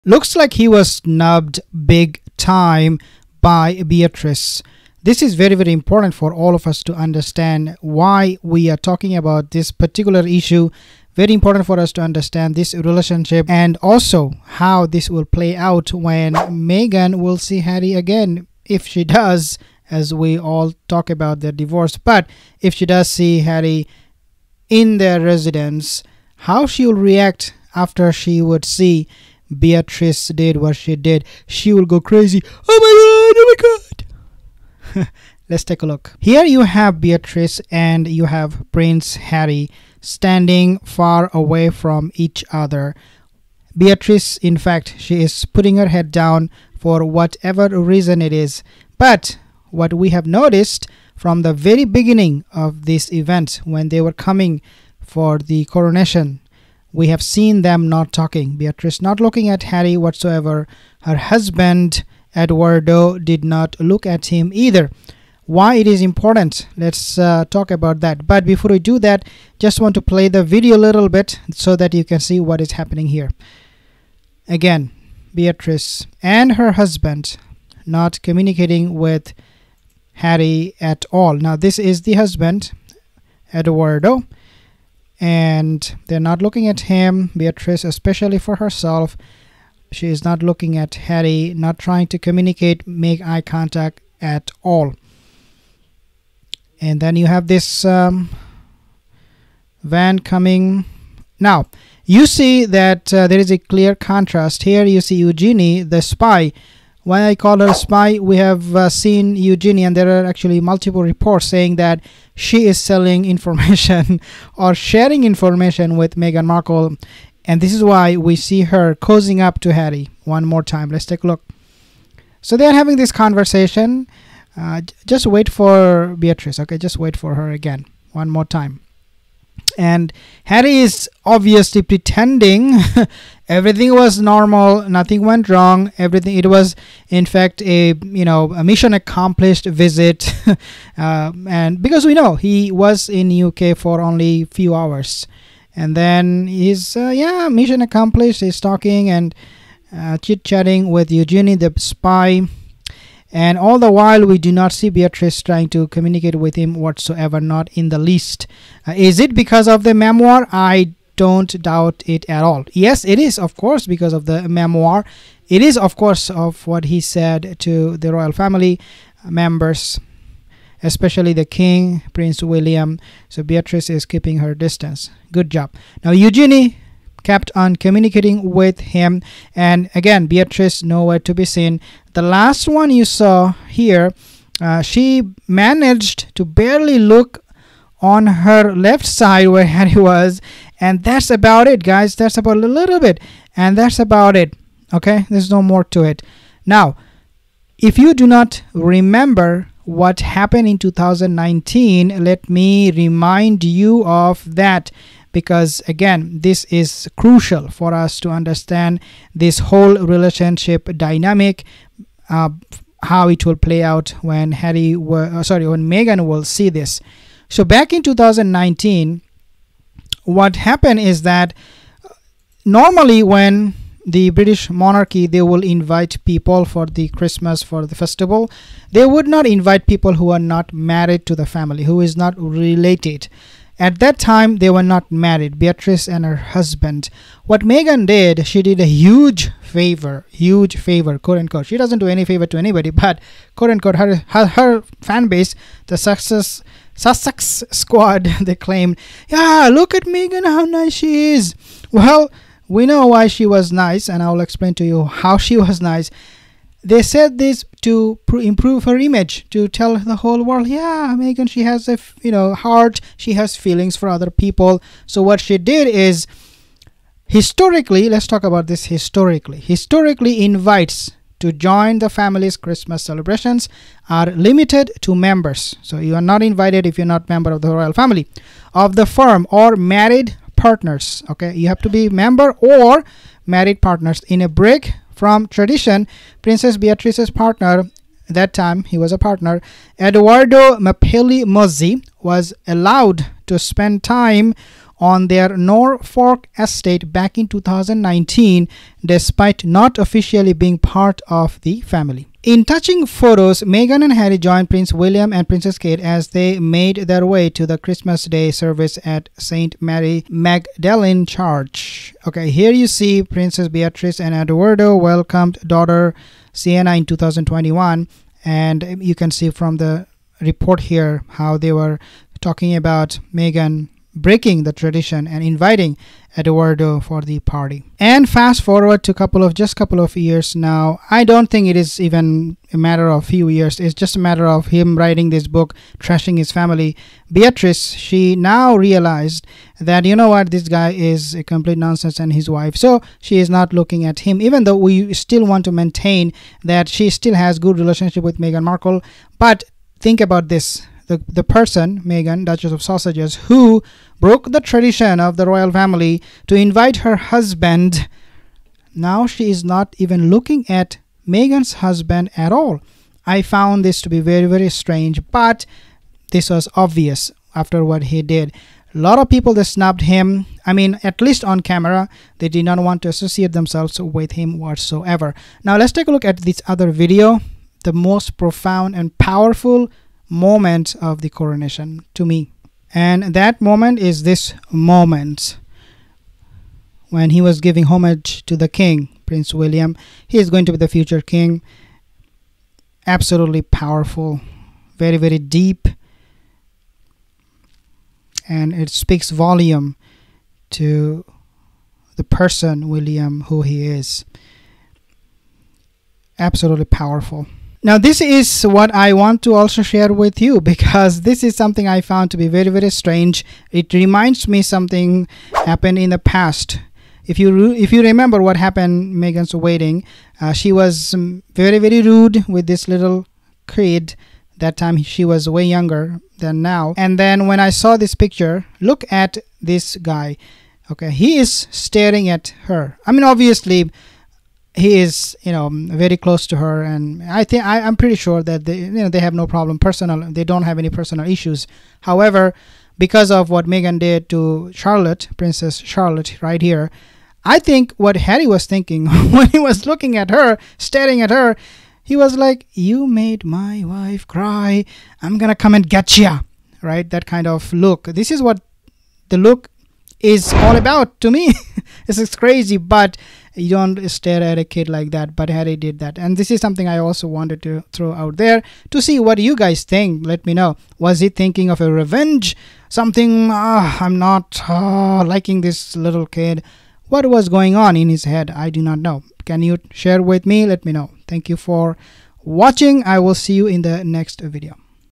What Harry was thinking STARING at Princess Charlotte & how he was SNUBBED again? (Markle/BRR)
Welcome to another Episode of British Royals Rise- Story of Meghan, Harry, Netflix, Spotify, Trevor, Prince Andrew, William